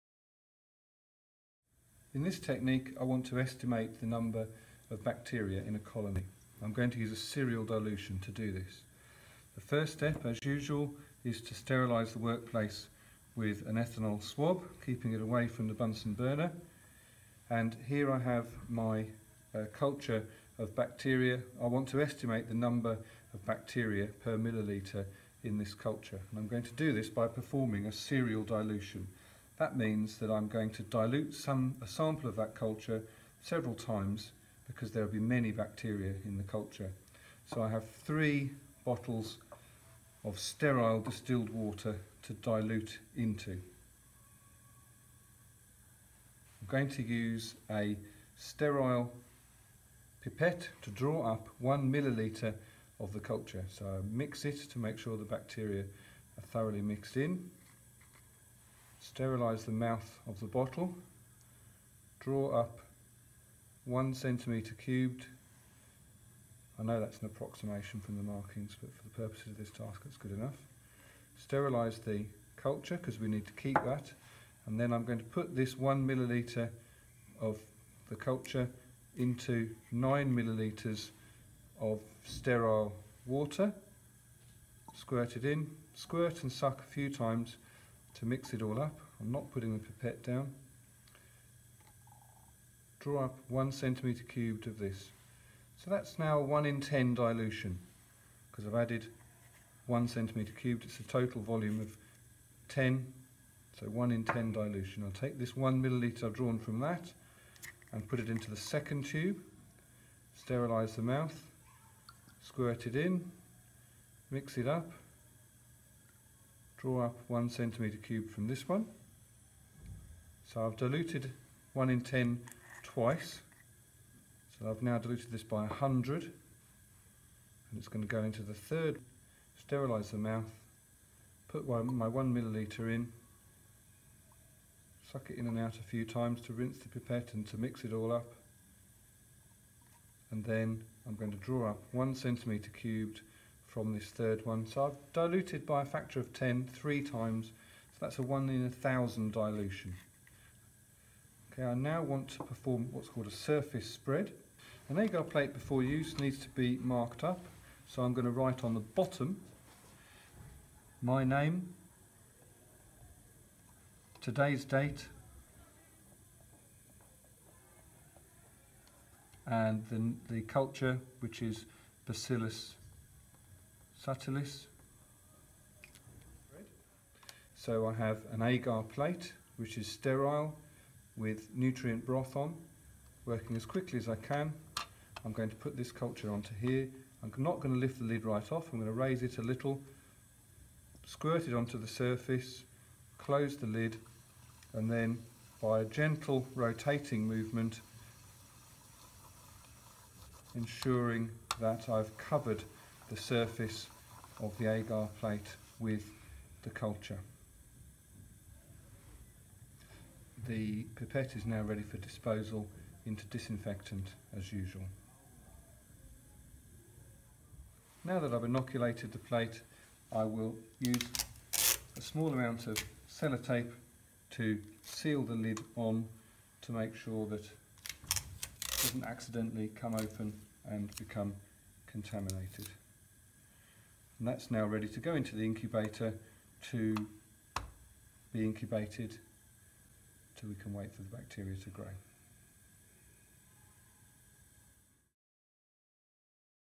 spread plate
Instructional video showing serial dilution of a bacterial culture and then making a spread plate